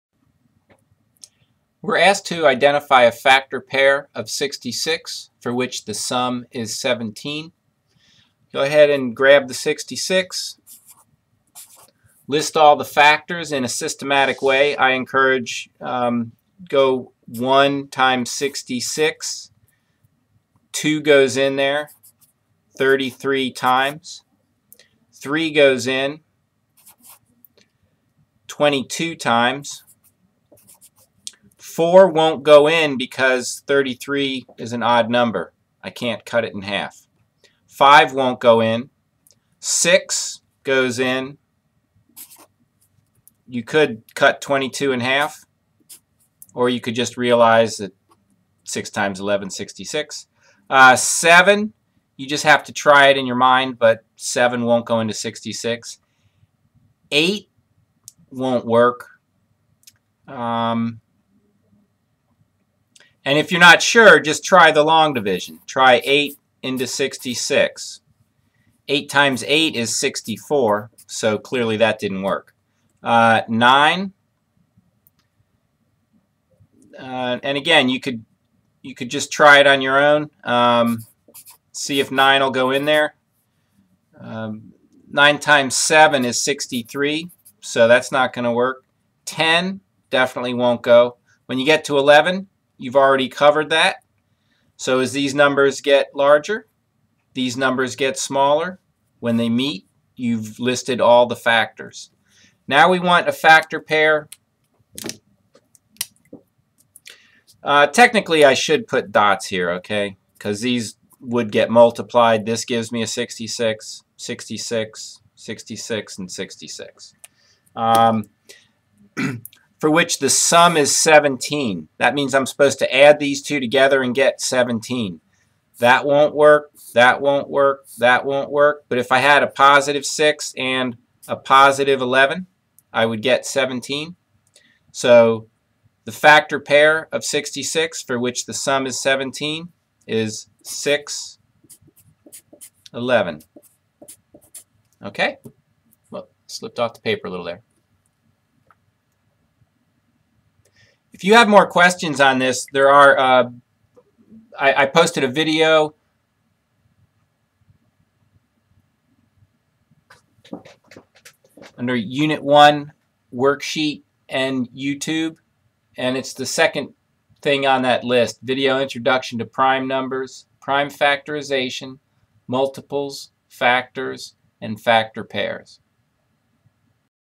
HW 018 U1 1.6 Factor Pair and Sum
I show how to list the factors of a number and then pick the factor pair that sums to a specified number.